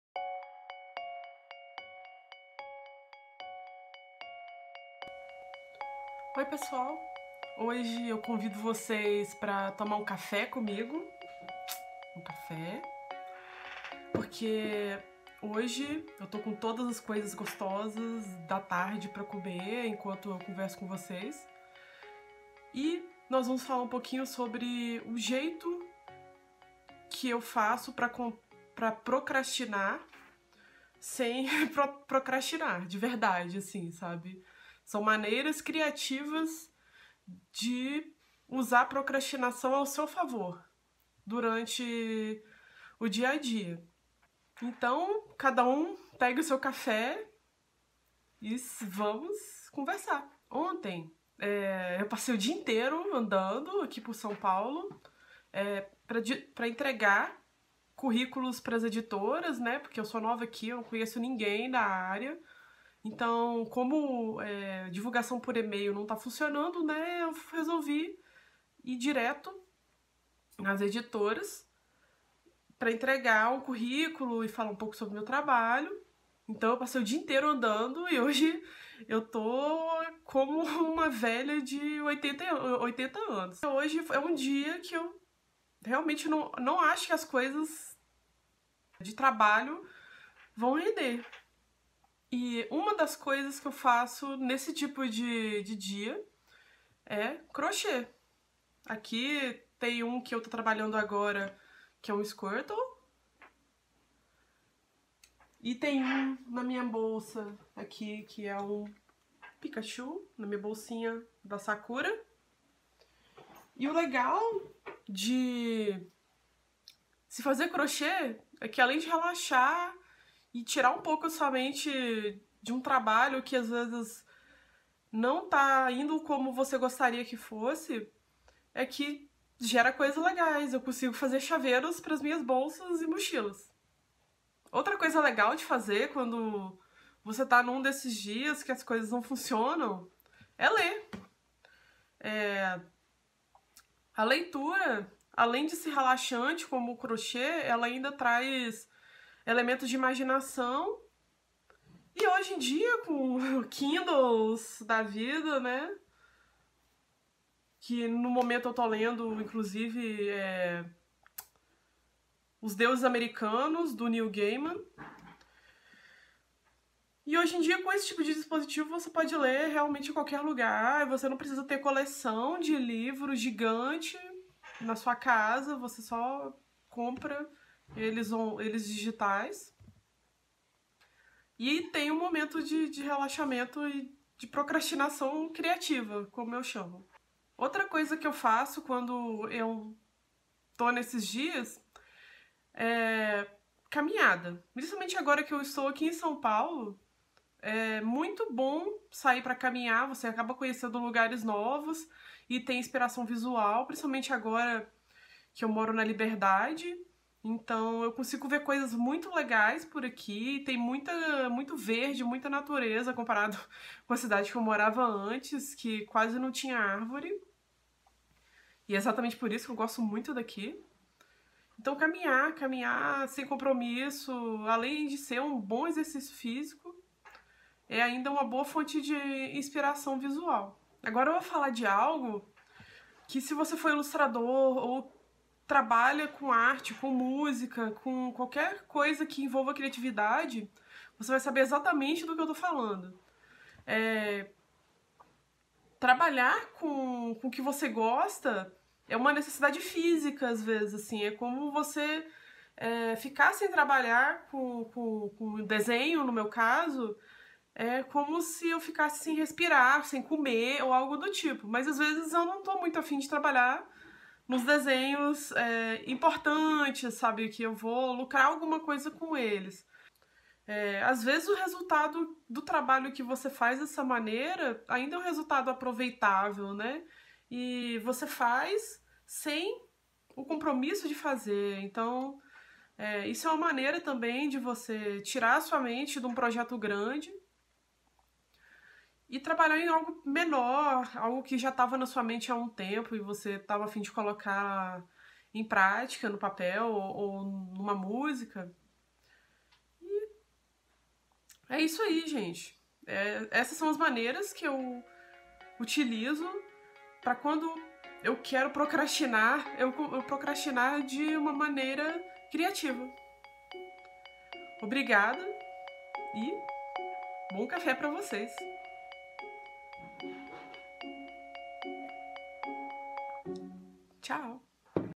Café com a Paula - Procrastinação / Coffee with Paula - Procrastination
PT

Oi pessoal! Neste vídeo eu falo sobre como são para mim, aqueles dias que os projetos simplesmente não querem progredir! Espero que gostem!

ENG
Hello everyone! In this video I talk about how it is for me those day when the progress just won't progress! I hope you like it! (There are english subs!)